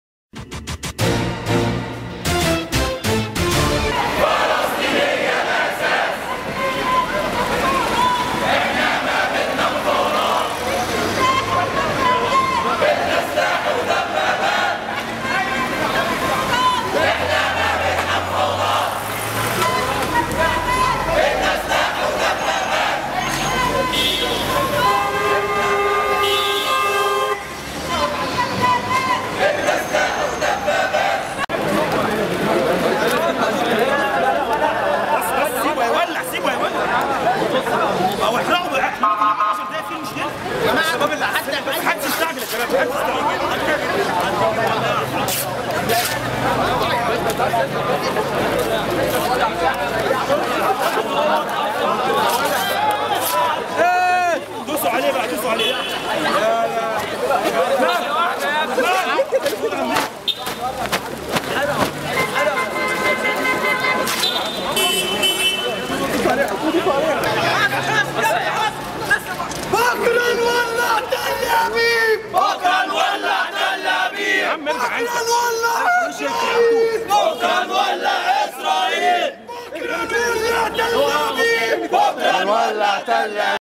حرق العلم الإسرائيلى أمام نقابة الصحفيين‎
نظم، منذ قليل، العشرات من النشطاء والمواطنين الغاضبين وقفة احتجاجية أمام نقابة الصحفيين احتجاجا على الممارسات الإسرائيلية وحرق الطفل الفلسطينى على الدوابشة.
ورددوا هتافات معادية للكيان الصهيونى "تسقط تسقط اسرائيل"، "يا إسرائيلى يا عميل إحنا أحسن جيل"،"هما كلاب جيل ورا جيل"، "بنعاديكى يا إسرائيل"، "فلسطين عربية رغم أنف الصهيونية".
ورفع المحتجون صور الطفل الفلسطينى وعائلته، وأحرقوا العلم الإسرائيلى